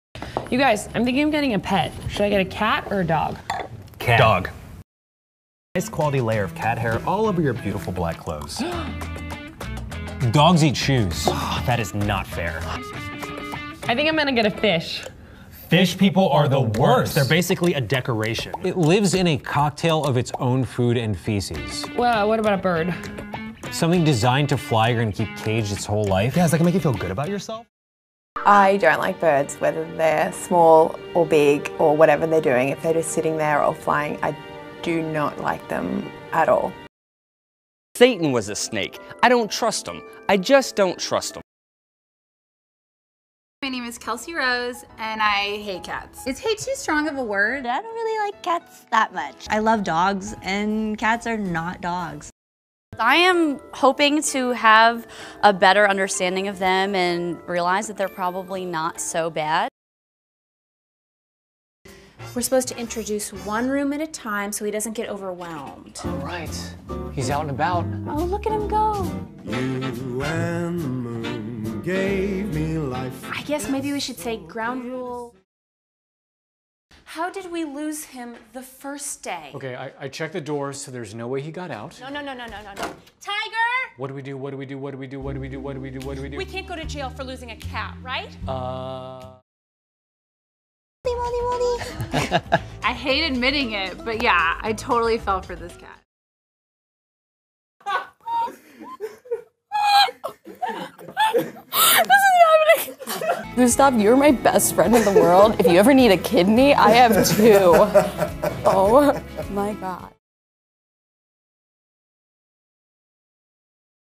Pet Swap
This is a demo of the reality show, Pet Swap, on Animal Planet, for team presentation project in CMS 306M, UT Austin